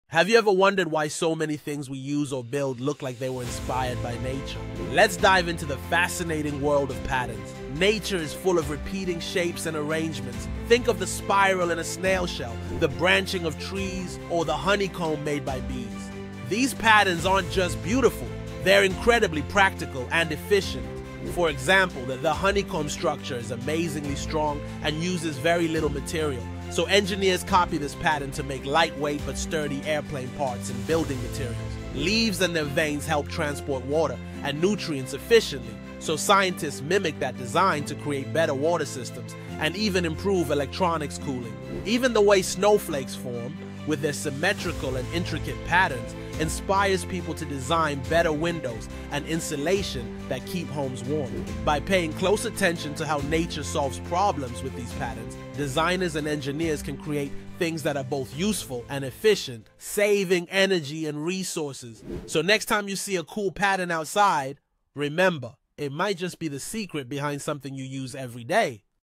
ELI5: How Do Patterns in Nature Inspire Designs?
Ever noticed how nature's patterns show up everywhere? Discover how leaves, shells, and even honeycombs shape the world around us!